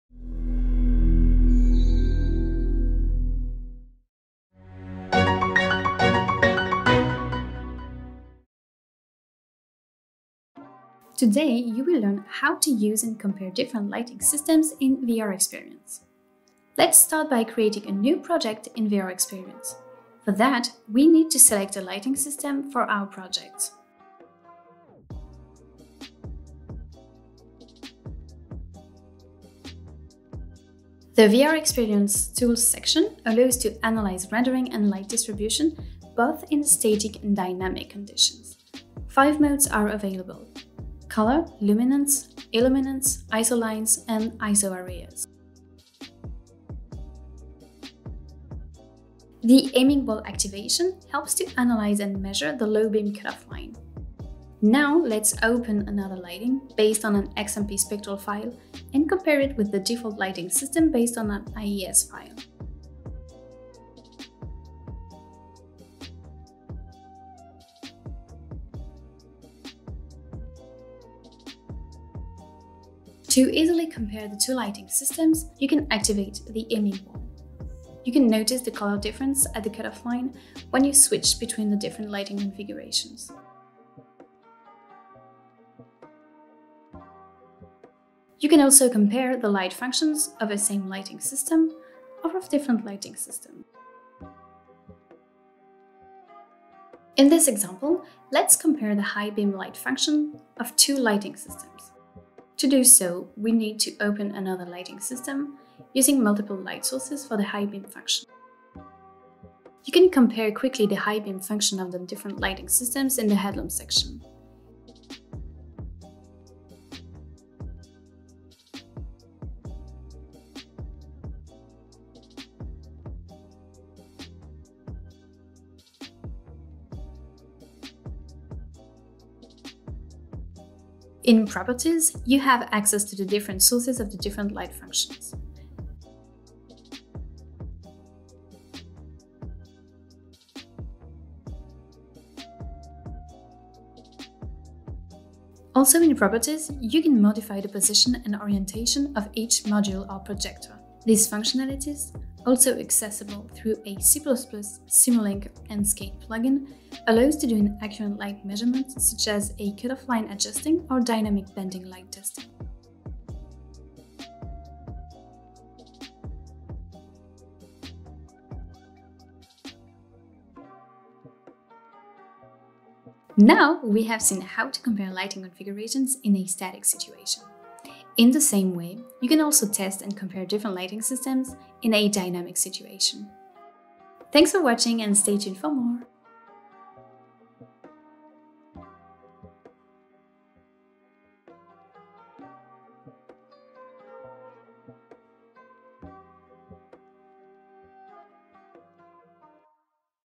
Ansys VRXPERIENCE Headlamp: Comparing different lighting systems
In this video, you'll learn how to use and compare different lighting systems in ANSYS VRXPERIENCE Headlamp. VRXPERIENCE reduces the need for night road tests by virtually assessing headlamp performance in real-time. It extends testing for intelligent lighting (AFS, ADB, matrix and pixel beam) and control law models to assess IIHS ratings.